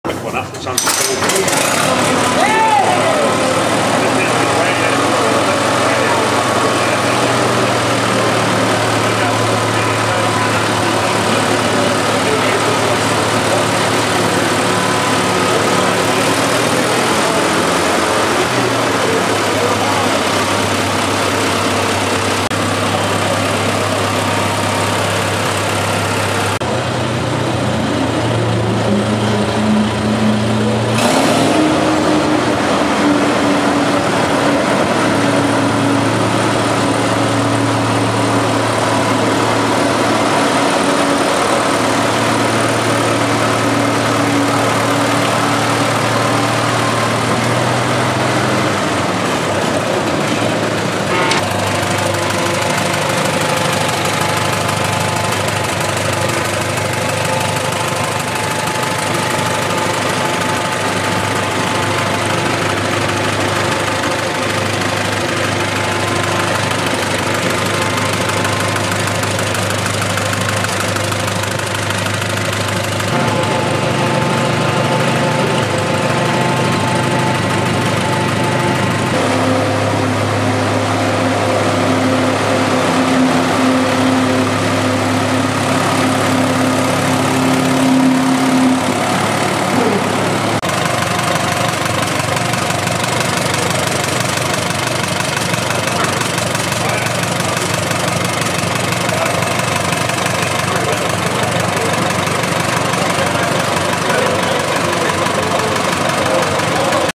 1943 Allis Chalmers Model U Tractor in Geraldine
1943 Allis Chalmers Model U Tractor, seen at the Geraldine Vintage Car & Machinery Museum Crank Up in Geraldine, in the Canterbury Region of New Zealand's South Island.